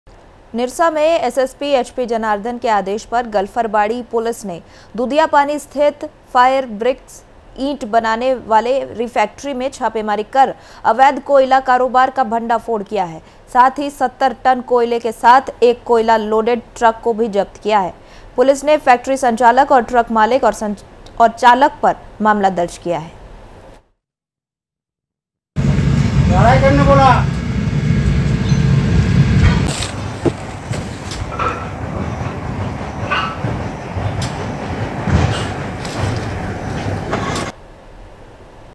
निरसा : दुधियापानी की रिफैक्ट्री में छापेमारी,पुलिस ने अवैध कोयला कारोबार का किया भंडाफोड़
Raftaar Media